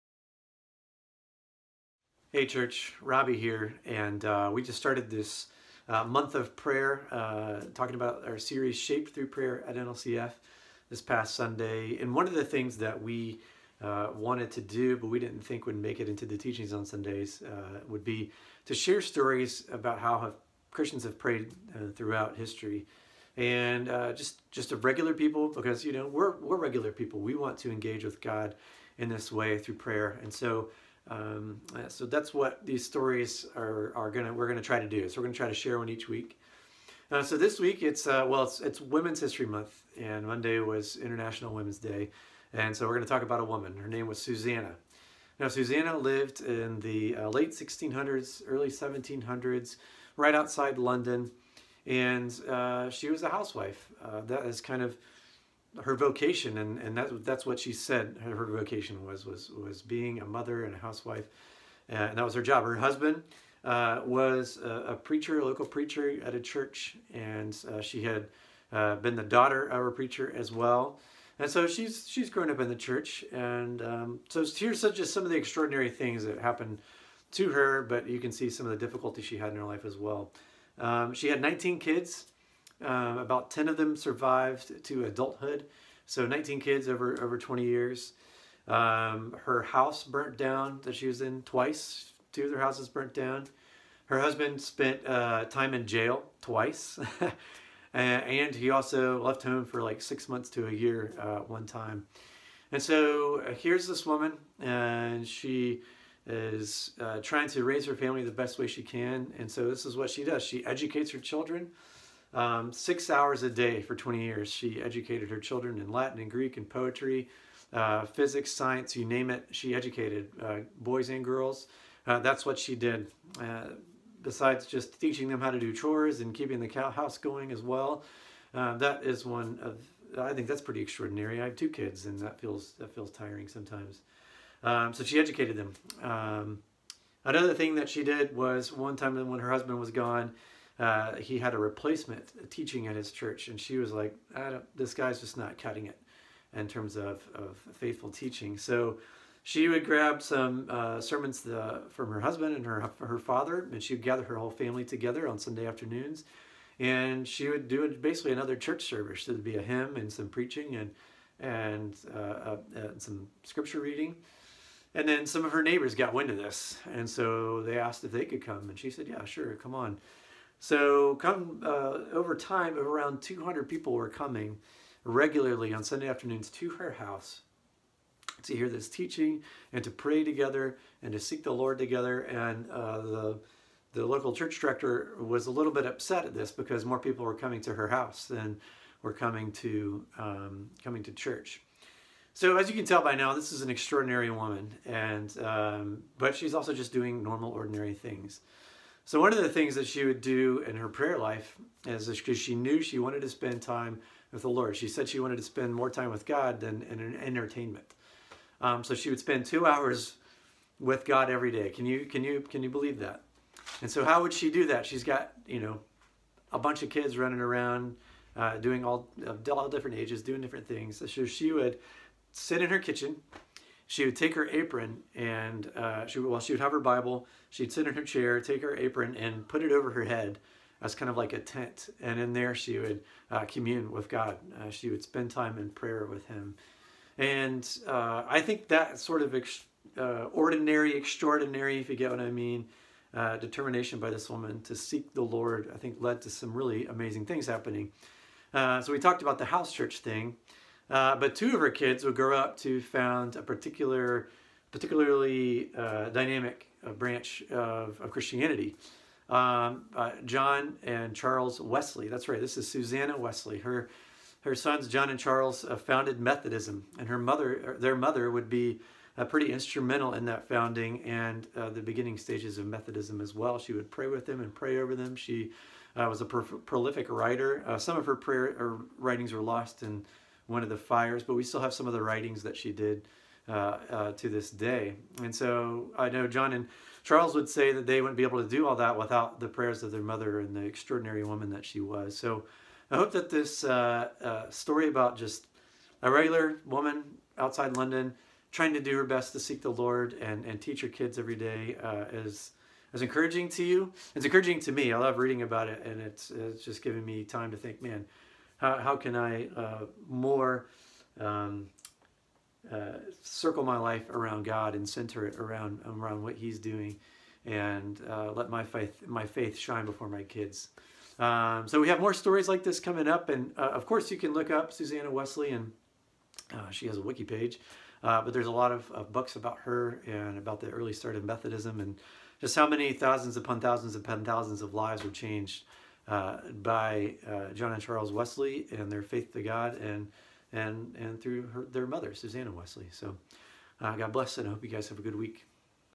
Shaped Through Prayer - Susanna Wesley
During our month long focus on prayer, we're going to be sharing stories about God doing extraordinary things through ordinary people. Today's focus? Susanna Wesley.